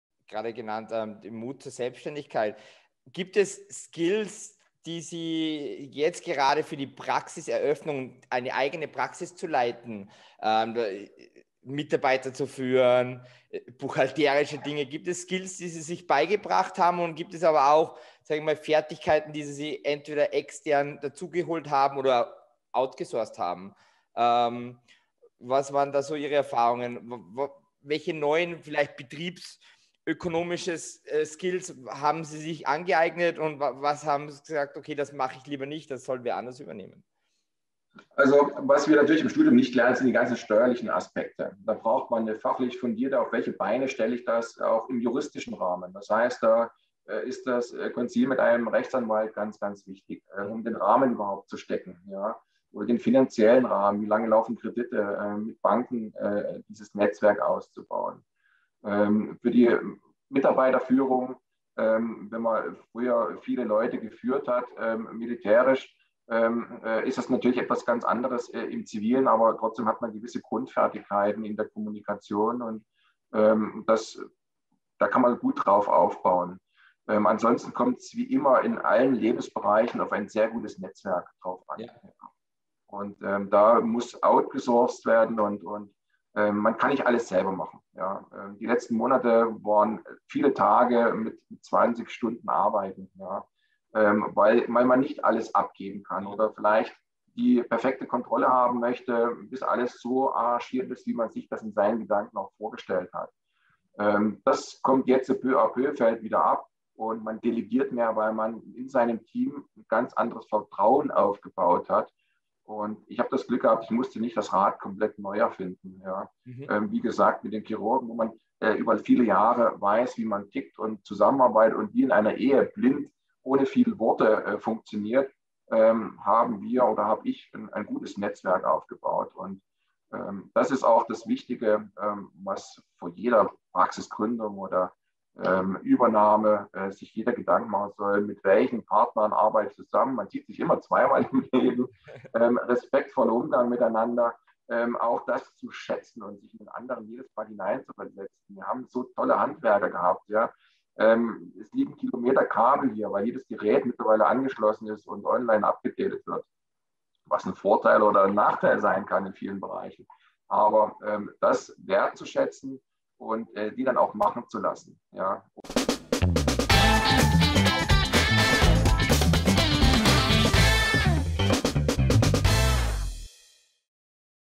Die Geschichte der Eröffnung meiner Zahnarztpraxis. Im Gespräch mit Dr. Lars Heidenreich
Dr. Lars Heidenreich hat vor kurzem seine private Zahnarztpraxis in Innsbruck, Österreich eröffnet.  Unser Team von Zircon Medical hat Dr. Heidenreich kürzlich in unserer Podcast-Serie zu Gast gehabt, um mit ihm über die Irrungen und Wirrungen bei der Eröffnung einer neuen Zahnarztpraxis zu sprechen.

Während des Podcasts haben wir die folgenden Themen diskutiert:

Die Herausforderungen bei der Eröffnung einer Zahnarztpraxis.
Die Bedeutung von Kommunikationsfähigkeiten bei der Führung einer Zahnarztpraxis.
Das Sammeln neuer Fähigkeiten und Erfahrungen bei der Eröffnung einer Zahnarztpraxis.
Tipps für Zahnärzte, die ihre eigene Praxis eröffnen.
Hören Sie rein, oder lesen Sie auch unseren spannenden Artikel zu diesem Thema: